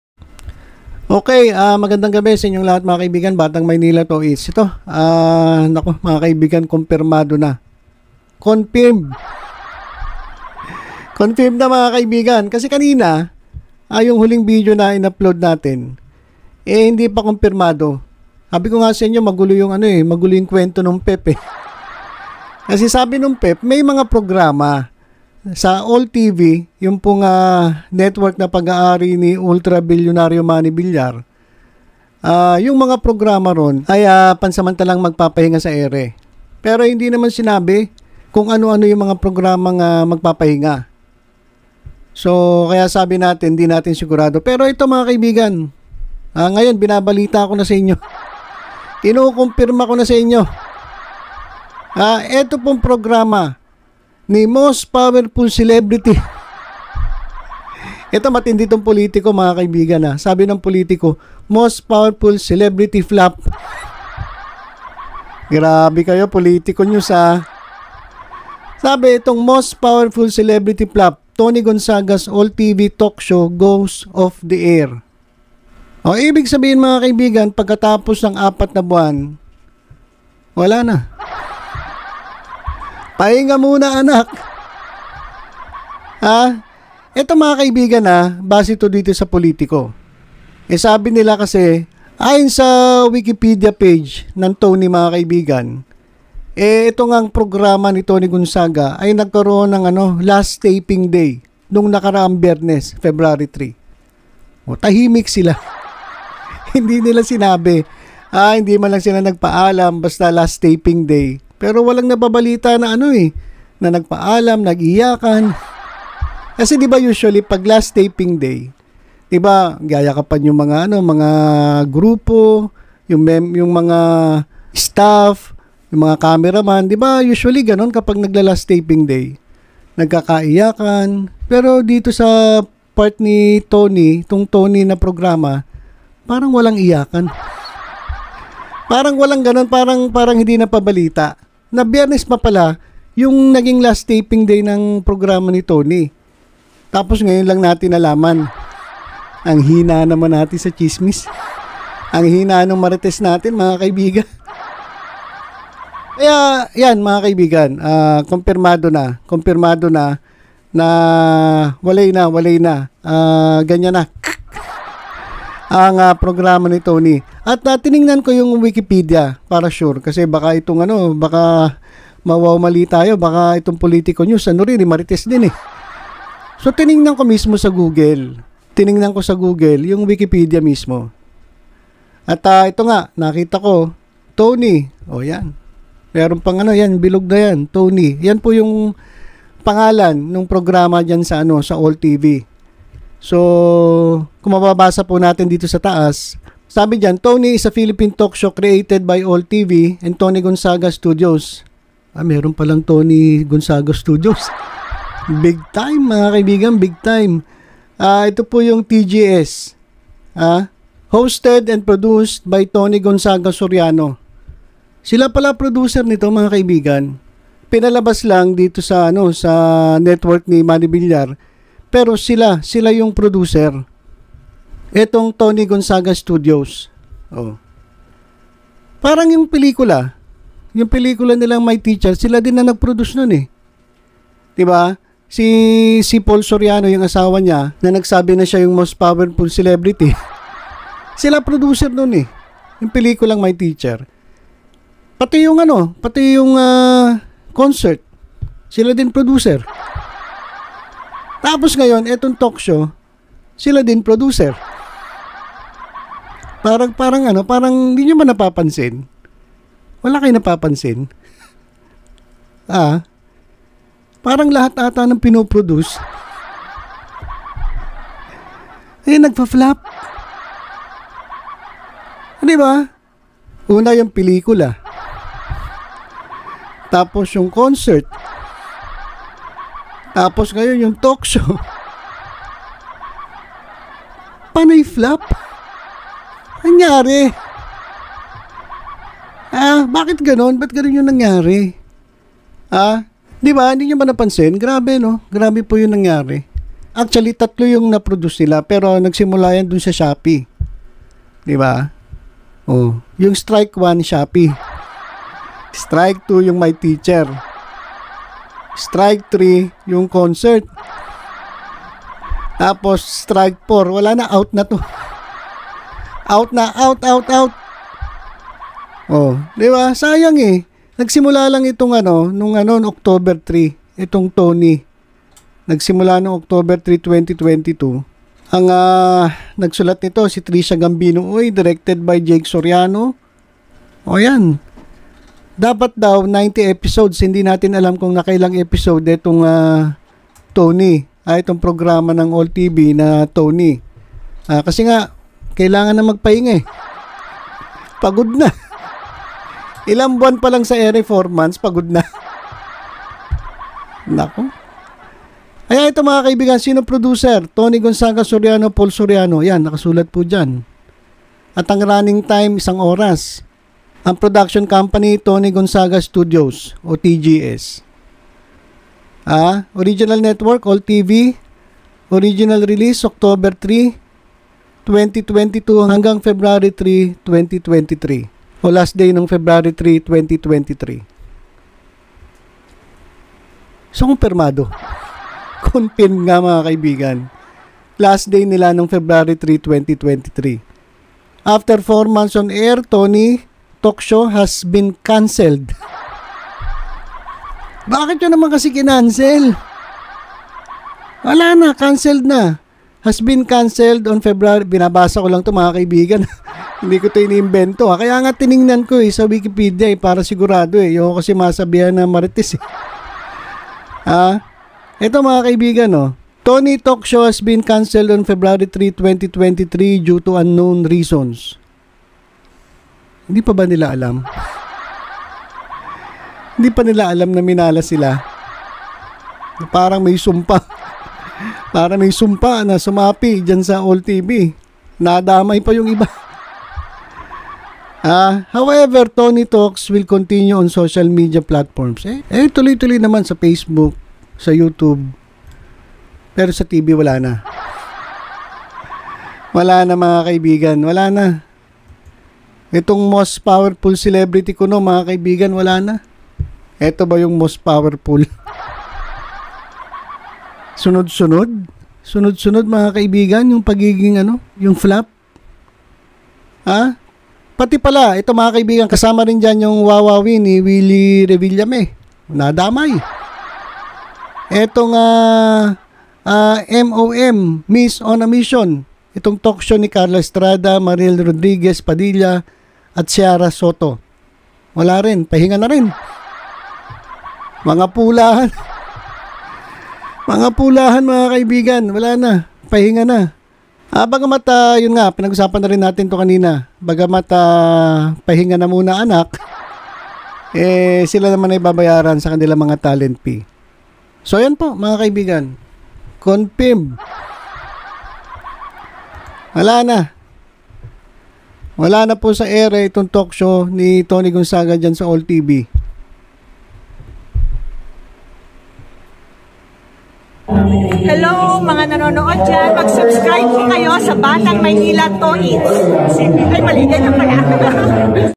CONFIRMED! GOODBYE 'TONI' NA TALAGA! CANCELLED NA ANG 'TONI TALK SHOW' SA ALL TV! ANYARE?!
CONTINUE TO SUPPORT MAYOR ISKO MORENO.